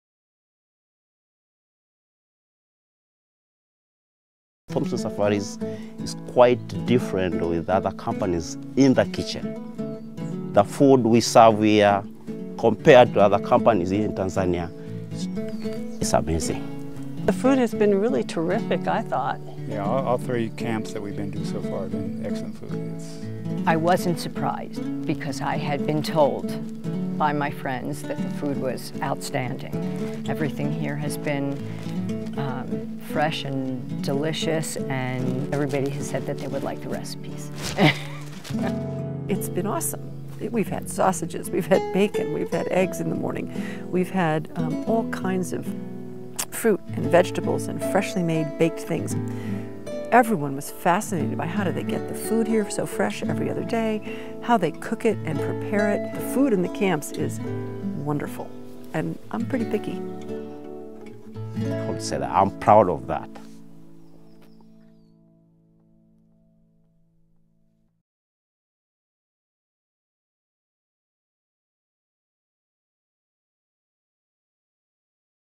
Safari Cuisine
Learn what our guests think about the food served in the Thomson Nyumba Camps where chefs prepare fresh soups, breads, entrees, and desserts daily.